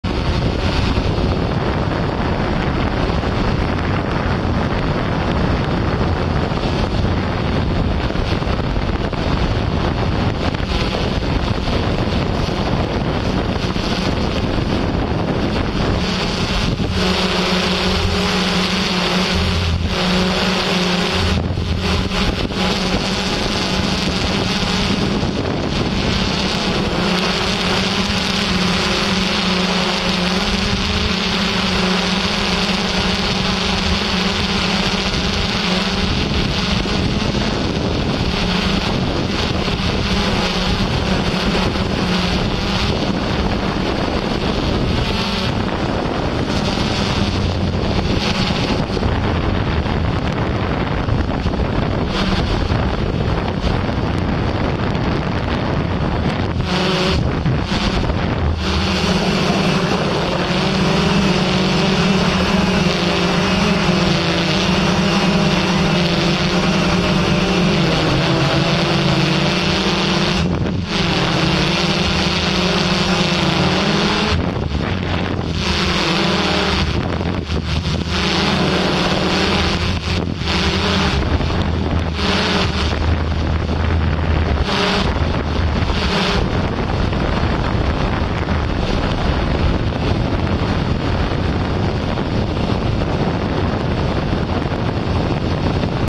Skateboard Electric Hub motor 100 mm urethane for MP83mm drone 4k
Follow me with the typhoon h in smart mode going approx 27mph on the skateboard.